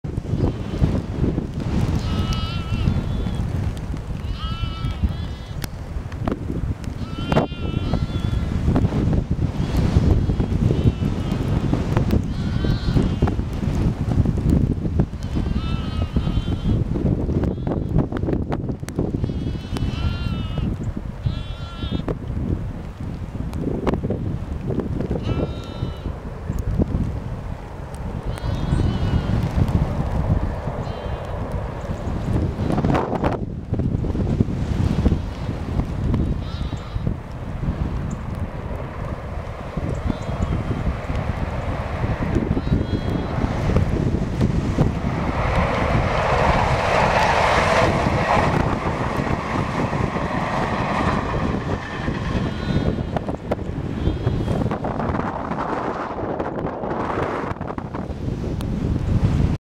Docker viaduct near Kendal
2 black 5s going north to take over the Great Britain railtour at Carlisle 15th April 2024. Of course 5 mins before and 5 mins after i took this there was bright sunshine and no wind!